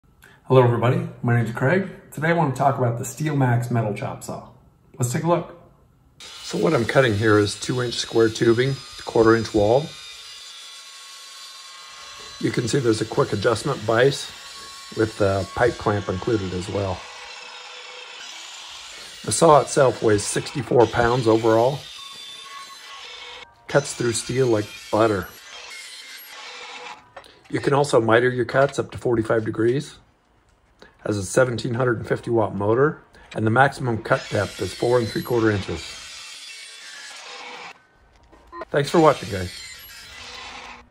Steelmax SM-S-14-C Metal Cutting Chop Saw, 14"
When a saw is engineered to work as efficiently as the Steelmax S14, your purchase pays immediate dividends. With its precise, fast, clean, burr-free, cool-to-touch cutting, further processing is eliminated, saving you expensive de-burring and further machining. Only Steelmax has a “flip plate” built onto its base that allows the S14 to securely hold round material, pipe and tubing so it doesn’t come loose or turn when being cut – which could damage the material or blade. And its exclusive heavy cast iron base reduces vibration to extend blade life and provide more precise cutting.